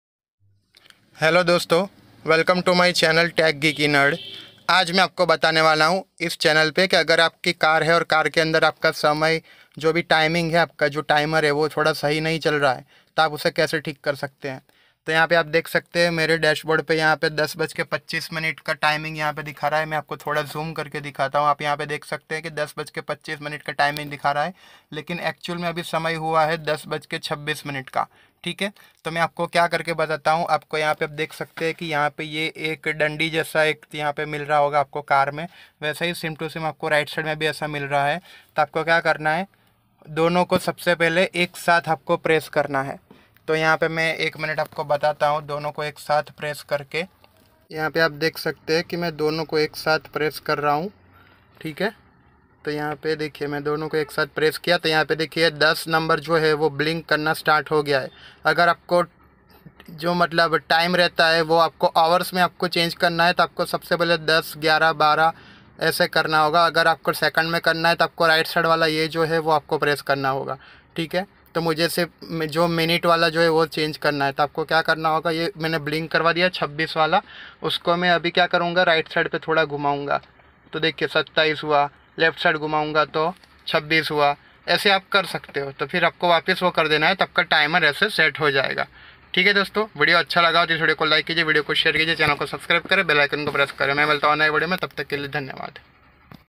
How to Set Time In Car Dashboard || Celerio || Timer || Pankil Shah
in this video we have shown you that how can you set timing correctly on car dashboard.